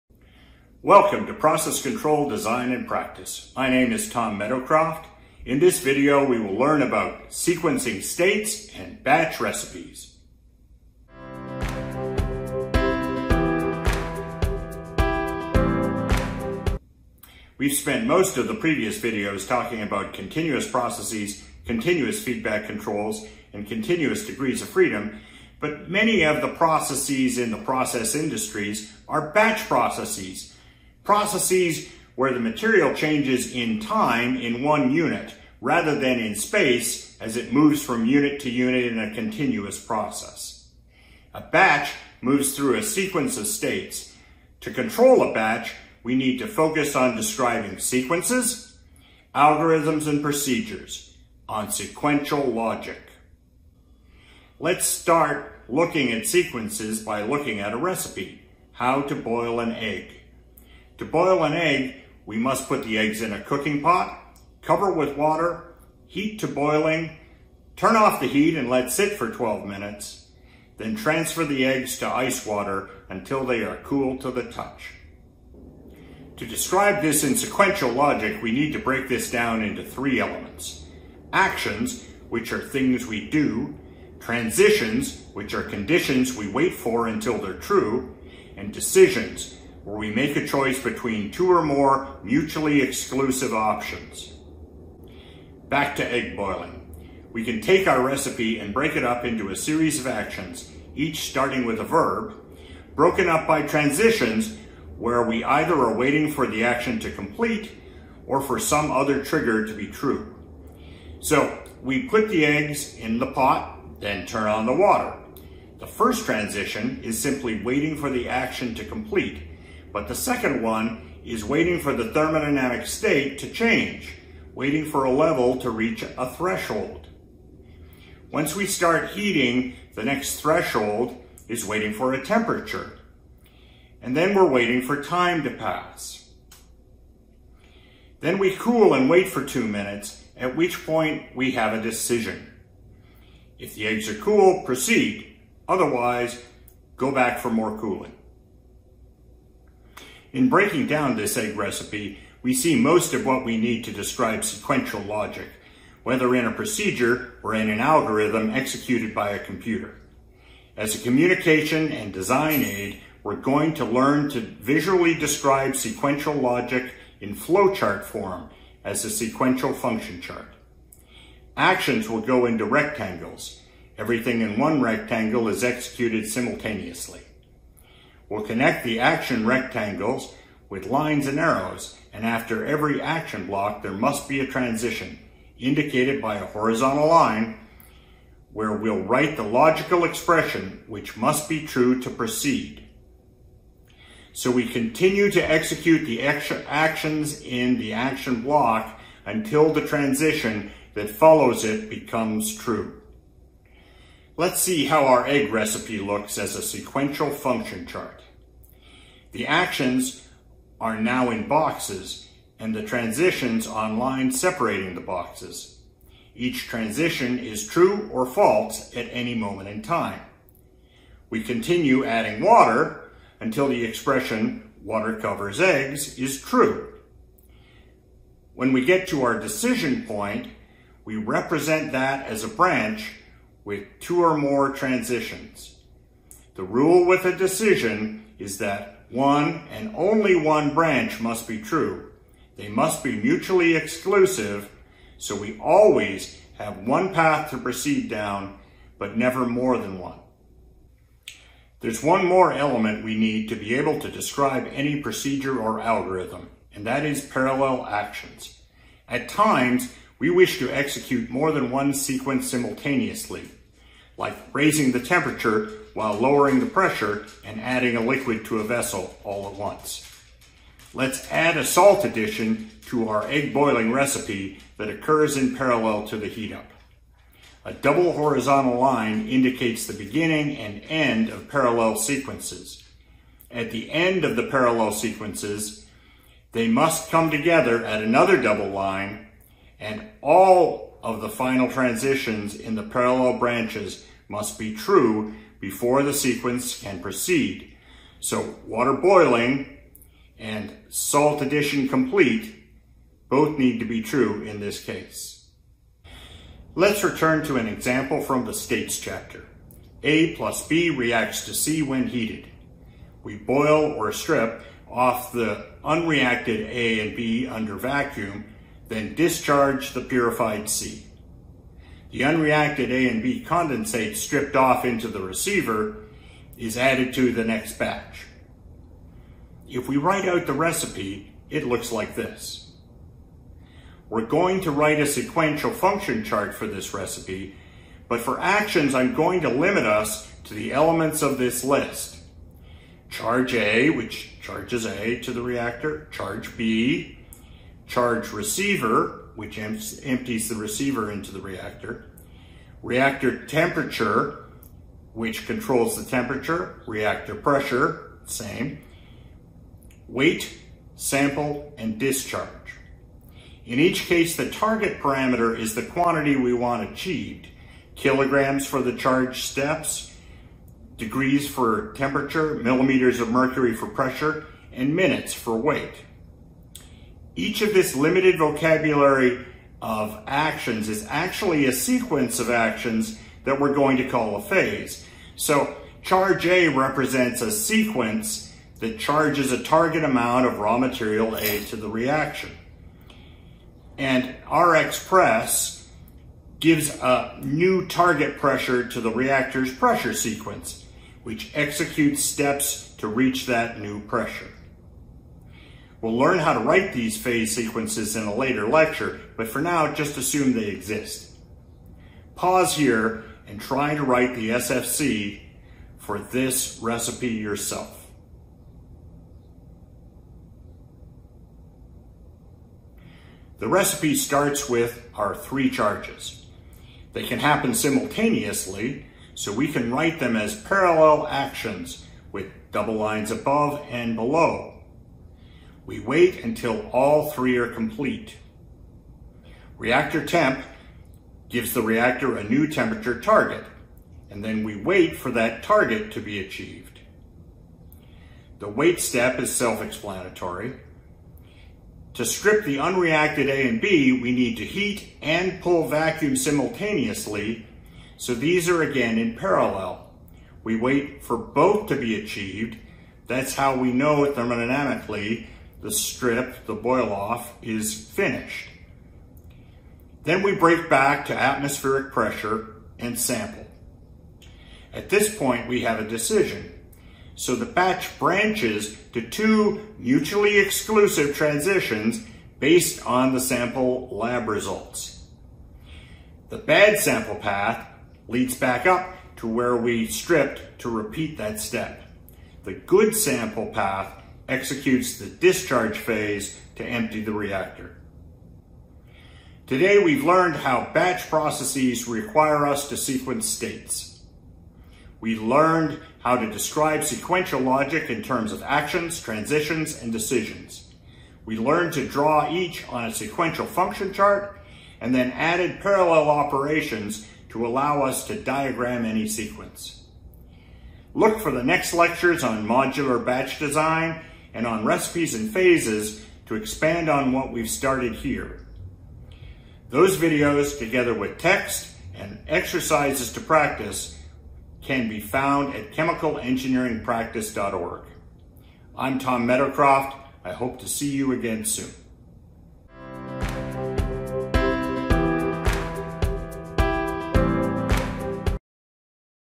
Sequential Logic (Batch Recipes)
#9 in the course "Process Control Design and Practice", a series of videos that teach about the design of automated processes.  Visit ChemicalEngineeringPractice.org for more videos (in order), accompanying text, and exercises.

 @Chemical Engineering Practice ​